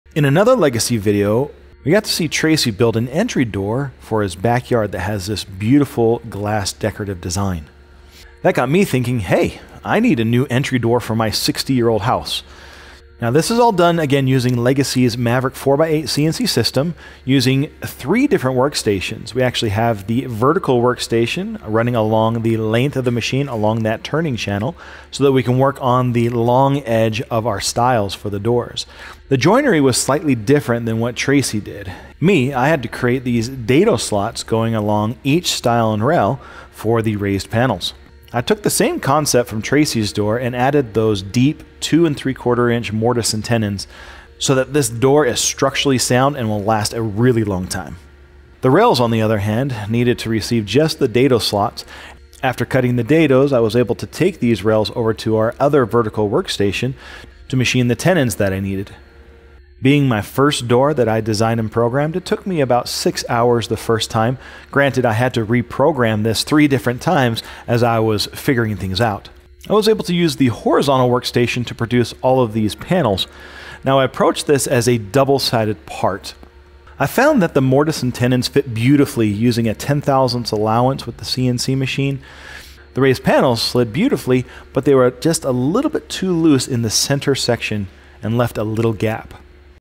How a CNC Can Build $3,400 Doors for $585 | Complete Door Build
In this video, we walk through the entire process of building a custom wooden entry door using Legacy CNC’s Maverick 4×8 system—featuring multiple workstations, deep mortise and tenon joinery, vertical clamping, and a complete time and profit analysis.

We show step by step:

✔ Vertical workstation setup (X-axis & Y-axis)
✔ Cutting precise 2.75" mortises and tenons
✔ Machining rails, stiles, and raised panels
✔ Lessons learned from warped panels
✔ Assembly, dry-fit, and final door evaluation
✔ Cost breakdown and profit potential for CNC door production
✔ How many doors you can realistically produce in a day, month, and year

Whether you’re a CNC owner, thinking of starting a small woodworking business, or simply love high-quality joinery, this video shows how vertical workholding on a CNC opens up enormous opportunities for production.

Software Used:
🎨 Aspire by Vectric for design & toolpaths